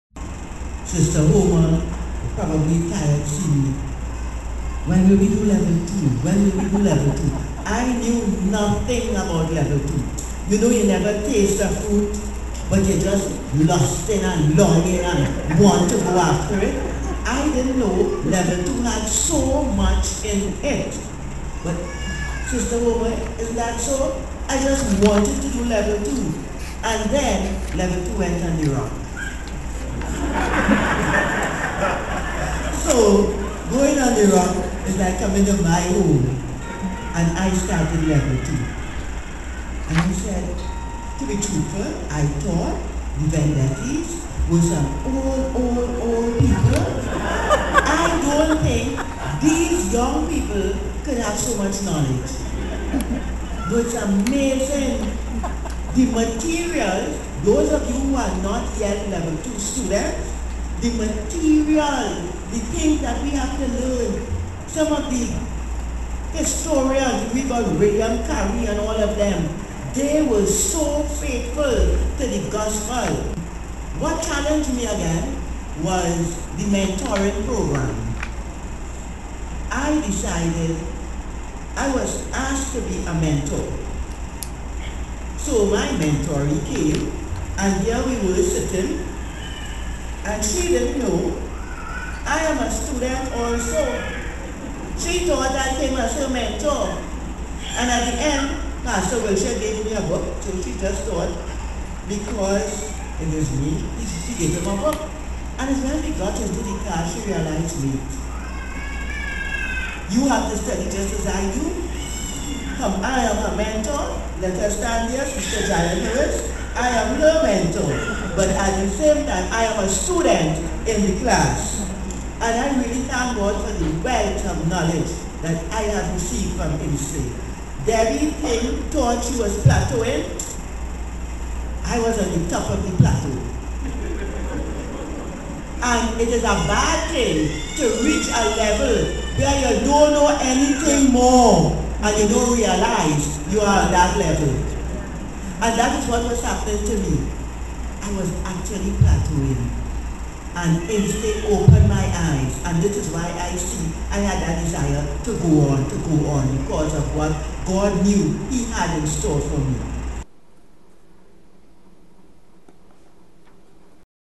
Testimony from Trinidad about Level Two
A woman from Trinidad shares her story about how she began to study in INSTE's Level Two program.